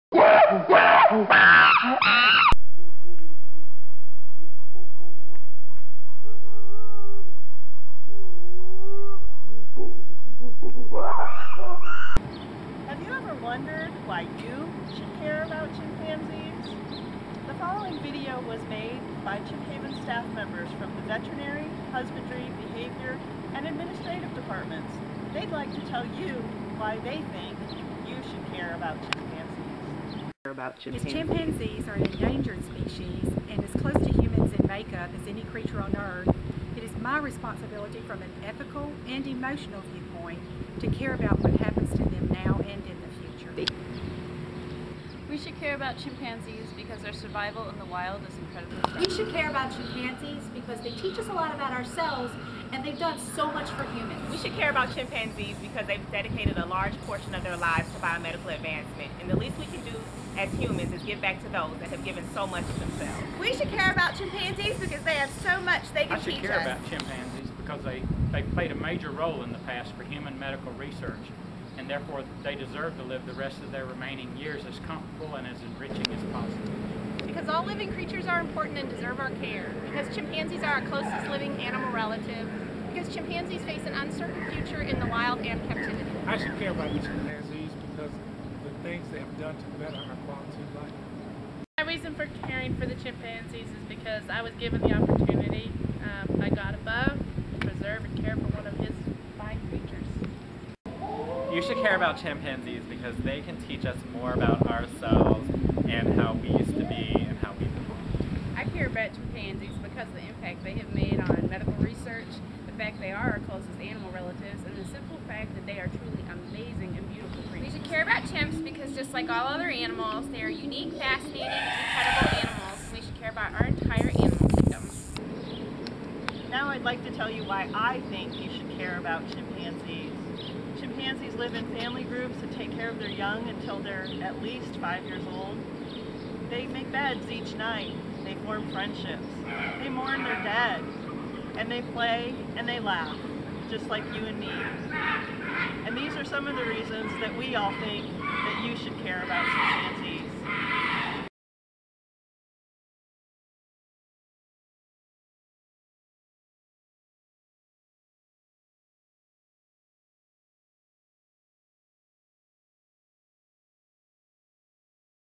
Why Chimp Haven Staff Members Care About Chimpanzees
The staff of Chimp Haven would like to tell you some of their diverse reasons for caring about chimpanzees and why they think that you should care too.